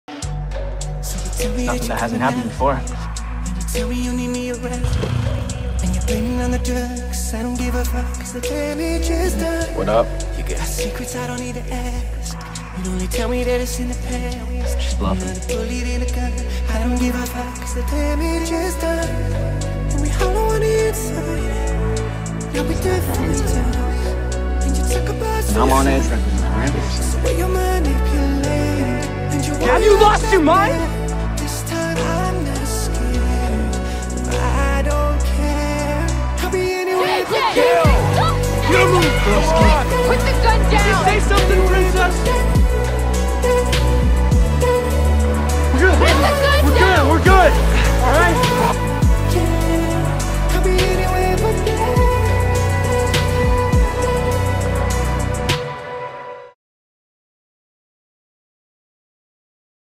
Carla & JJ
Please watch in 𝟏𝟎𝟖𝟎𝐩𝐇𝐃
hope everyone enjoys this random, little ship idea -  an extremely toxic obsession on both ends. mind games, non stops mind games. Enjoy!